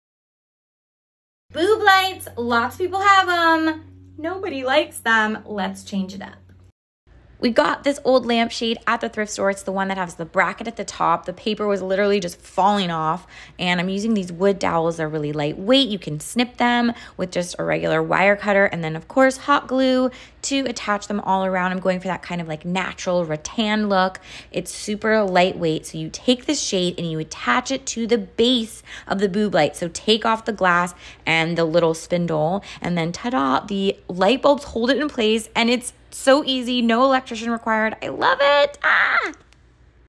Do you suffer from b🍈🍈b lights?
We replaced our boob lights with this DIY'd boho overhead fixture made out of an old lamp shade we got at the thrift store + wooden dowels.